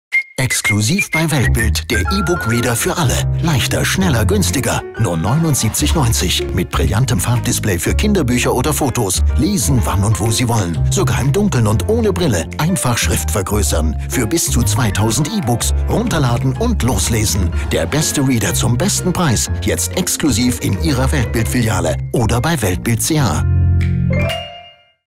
Weltbild TV-Spot: Weltbild eBook Reader 3.0
Der beste eBook Reader zum besten Preis! Der einfache eBook-Einstieg: leichter, schneller, günstiger - mit vielen tollen Funktionen! Exklusiv bei uns!